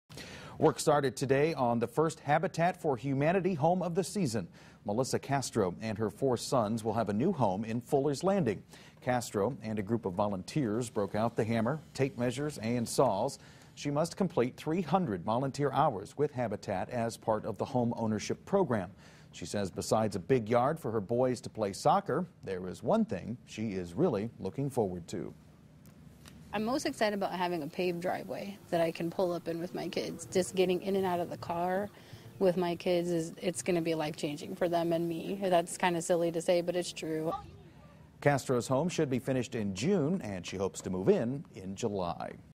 Habitat for Humanity starts building first home of the season
The new home is being built at Fuller's Landing.